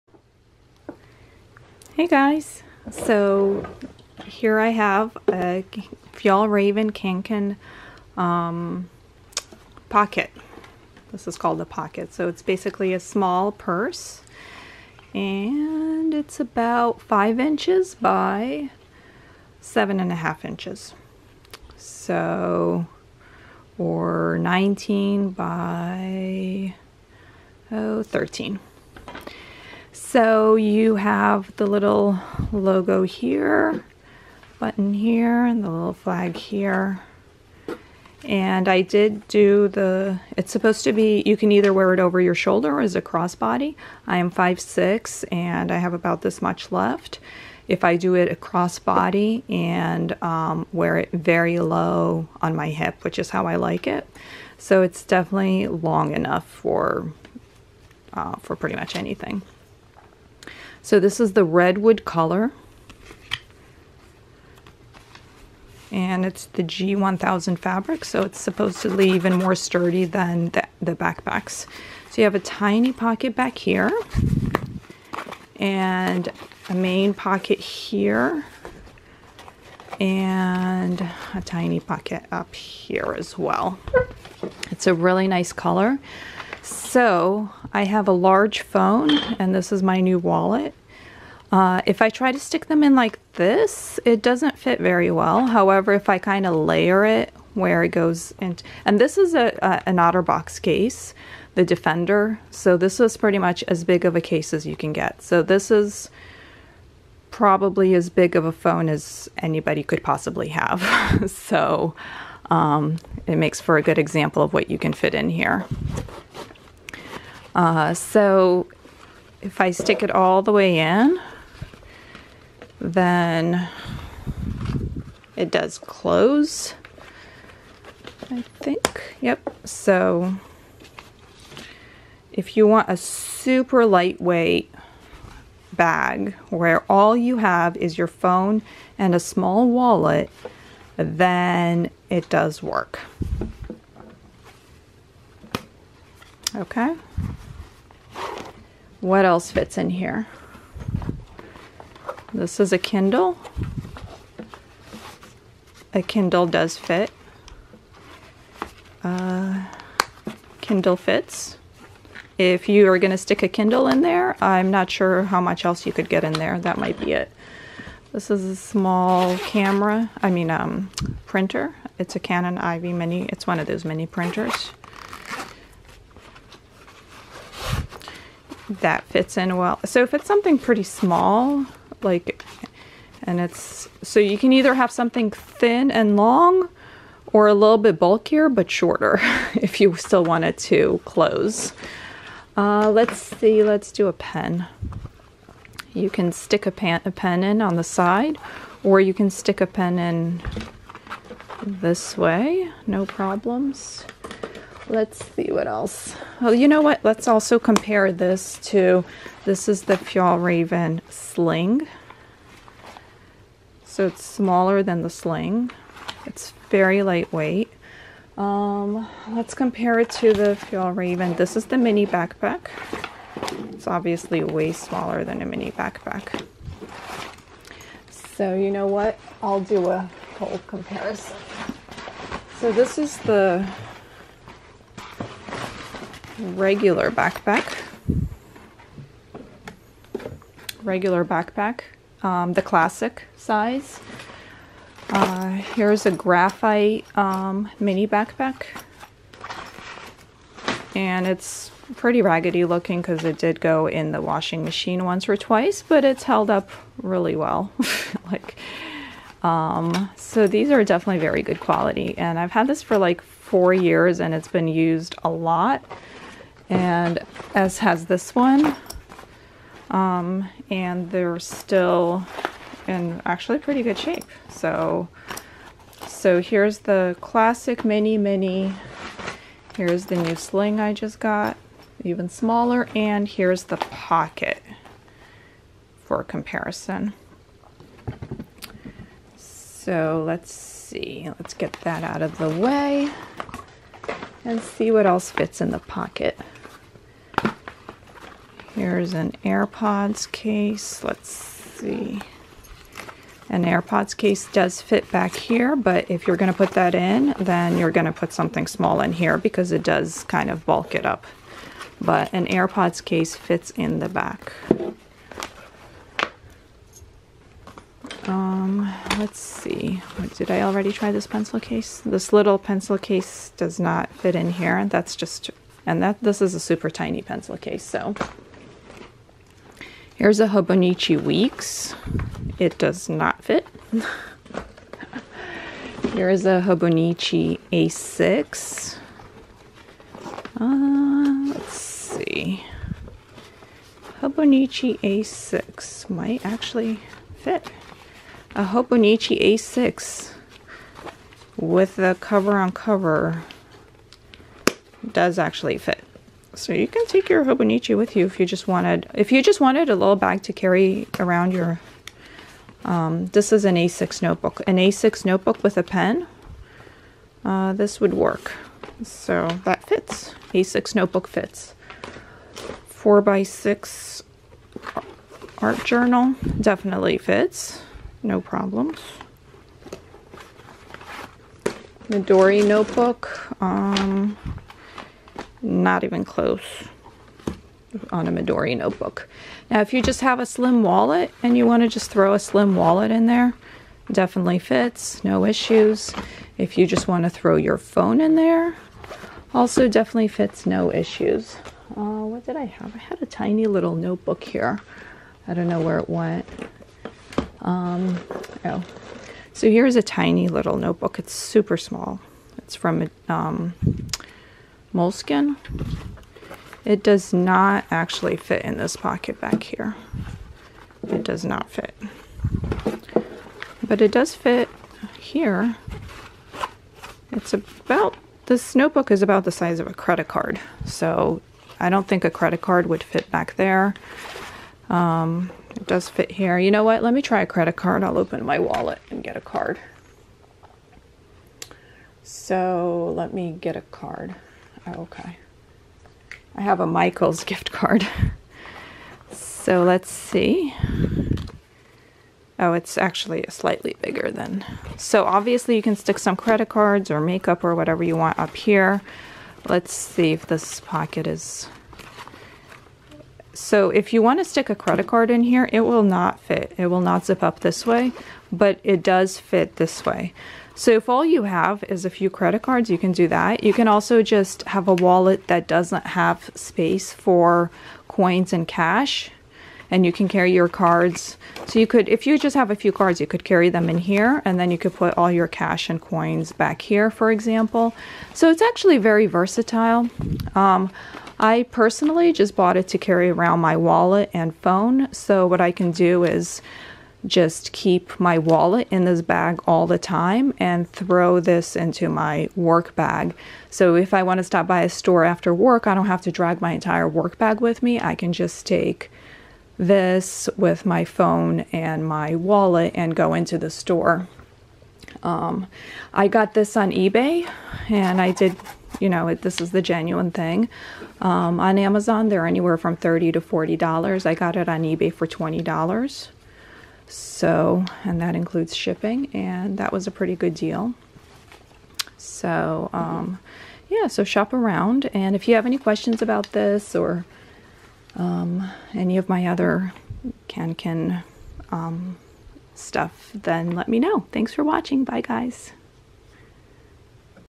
Fjallraven Kanken Pocket review + What's in my bag
Overview of my new Fjallraven Kanken pocket, plus what fits inside (journals, midori, traveler's notebook, hobonichi, phone and wallet, etc), and what I plan to use it for. Also comparing it to other Fjallraven bags: classic backpack, sling, mini backpacks. I've also done review videos of the mini backpacks and the sling.